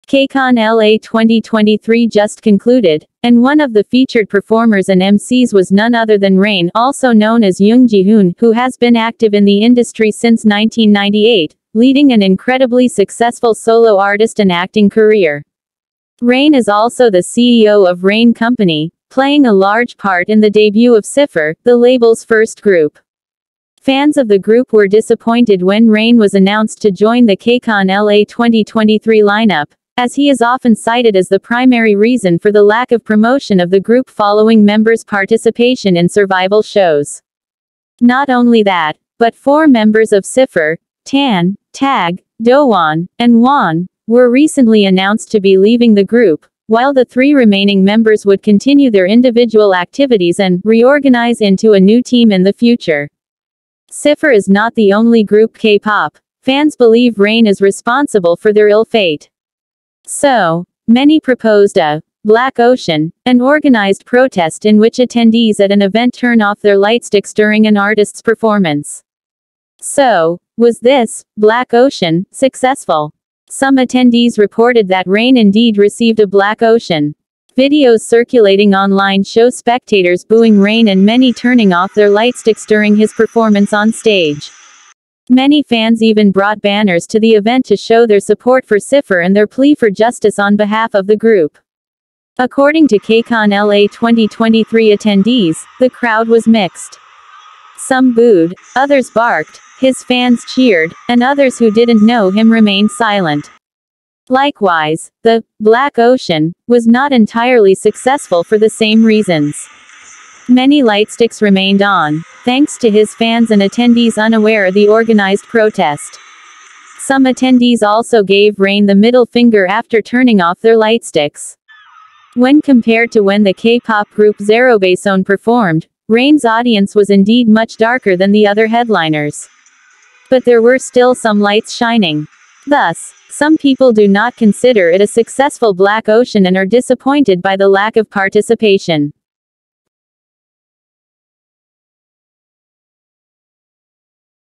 Did RAIN Really Receive A Black Ocean And Get Booed At “KCON LA 2023”?
Did RAIN Really Receive A Black Ocean
 And Get Booed At “KCON LA 2023”?

KCON LA 2023 just concluded, and one of the featured performers and emcees was none other than Rain (also known as Jung Jihoon), who has been active in the industry since 1998, leading an incredibly successful solo artist and acting career.

Rain is also the CEO of RAIN Company, playing a large part in the debut of Ciipher, the label’s first group. Fans of the group were disappointed when Rain was announced to join the KCON LA 2023 lineup, as he is often cited as the primary reason for the lack of promotion of the group following members’ participation in survival shows.